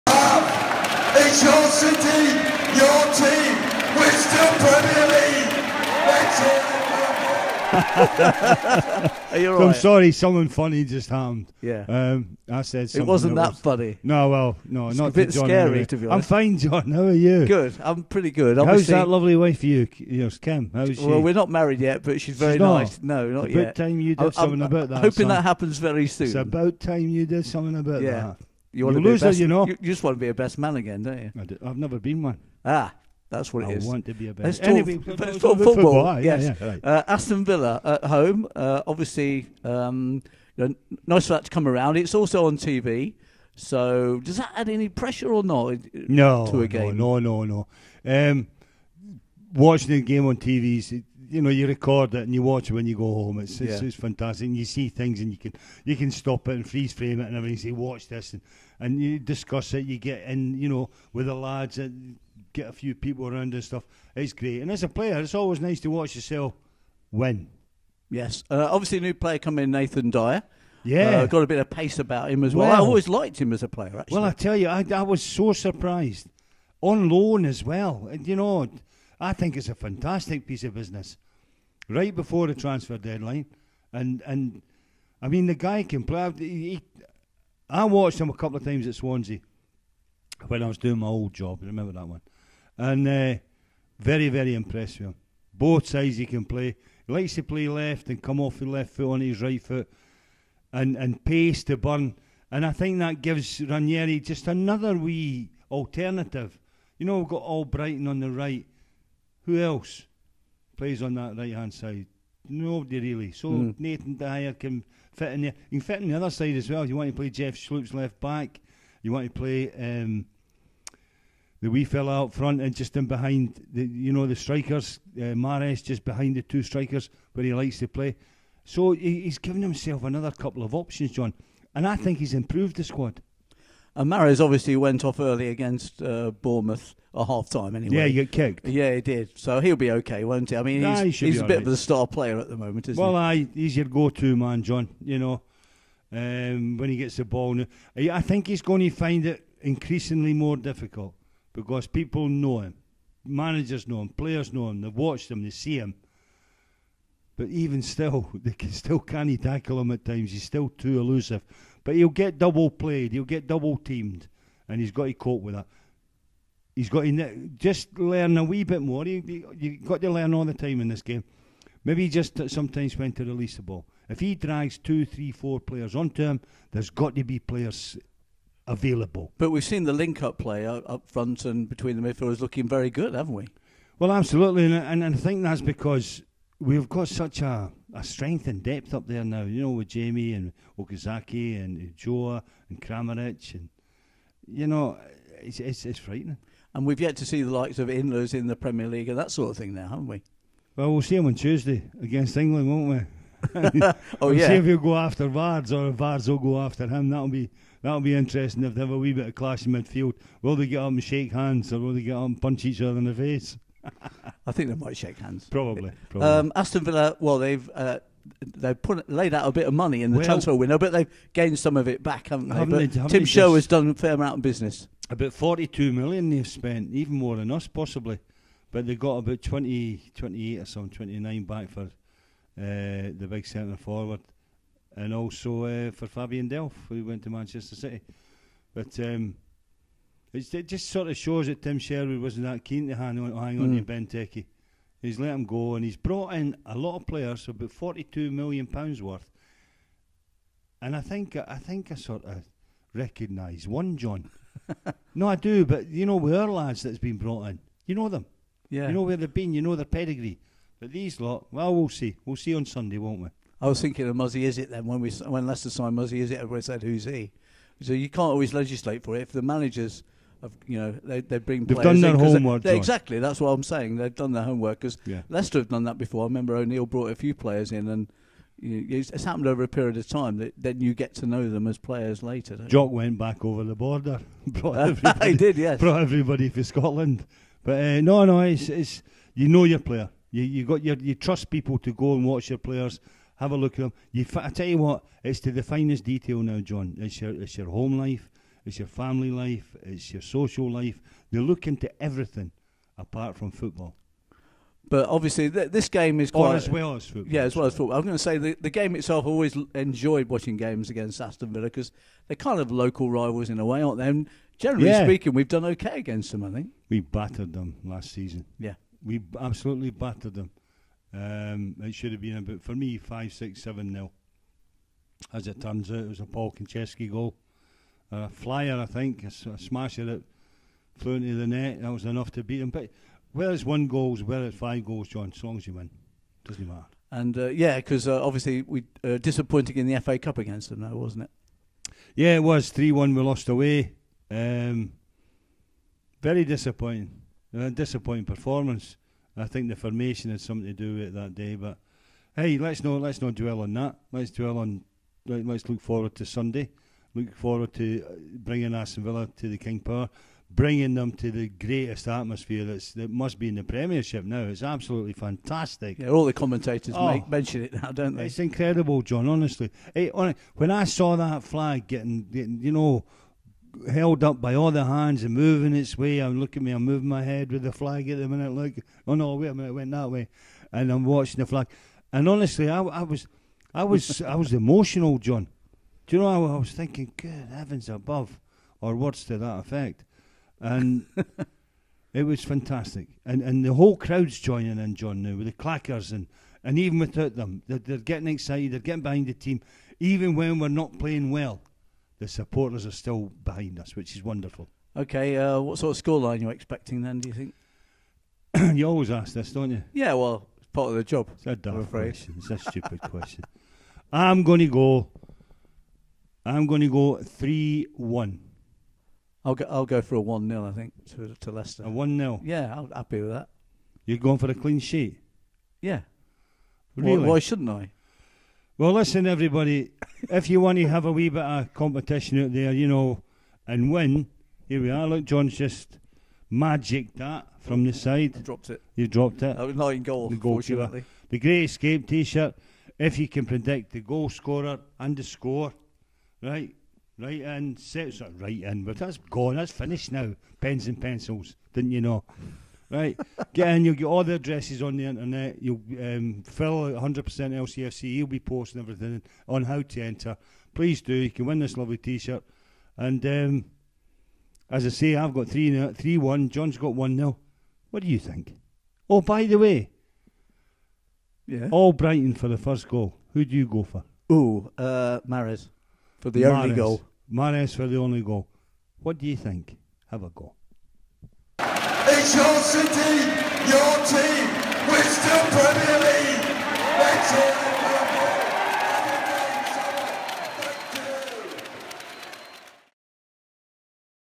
Leicester v Villa Pre Match Show
Former Leicester City legend Alan Young chats with presenter John Sinclair about the next match for Leicester City, which sees local rivals Aston Villa arrive at the King Power stadium for the Sunday LIVE game on Sky TV.

Can City carry on their great start to the season ?

Sinky and Youngy chat all things Leicester City